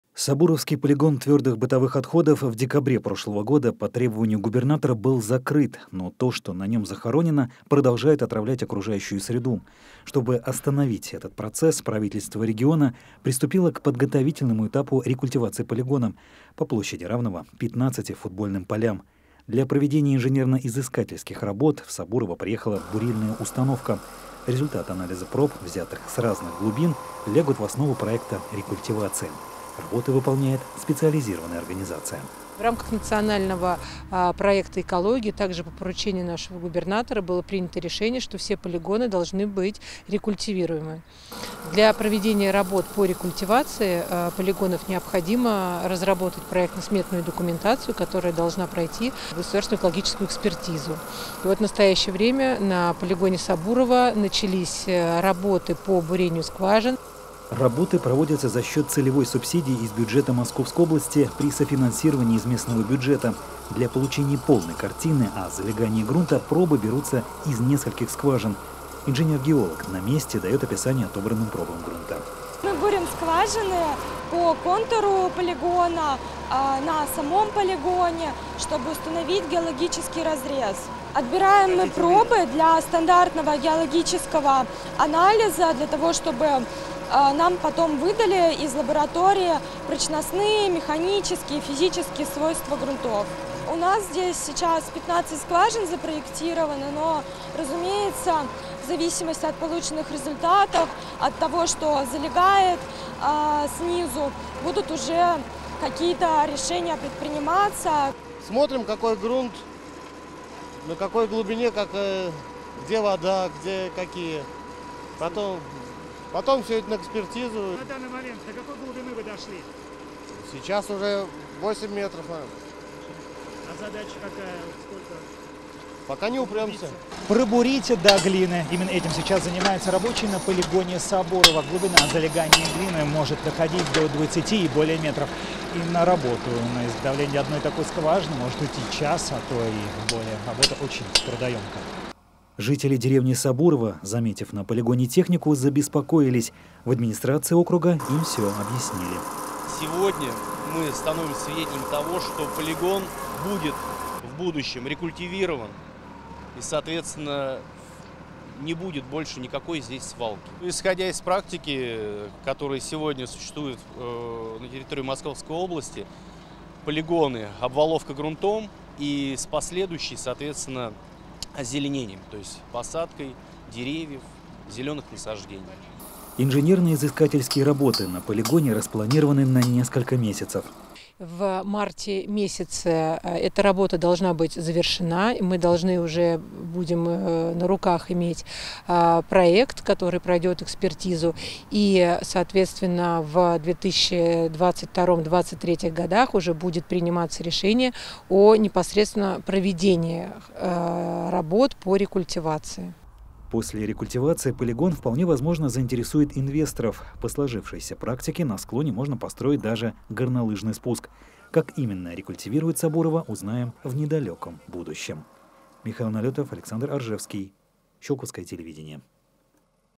В Сабурово ведётся подготовка к рекультивации свалки
От мусорной свалки до горнолыжного спуска. В деревне Сабурово приступили к подготовительному этапу рекультивации полигона твёрдых бытовых отходов – инженерно-изыскательским работам. На основе собранных сведений о залегании слоёв грунта будут разработаны мероприятия по снижению вреда накопленных отходов на окружающую среду. Подробности смотрите в нашем сюжете.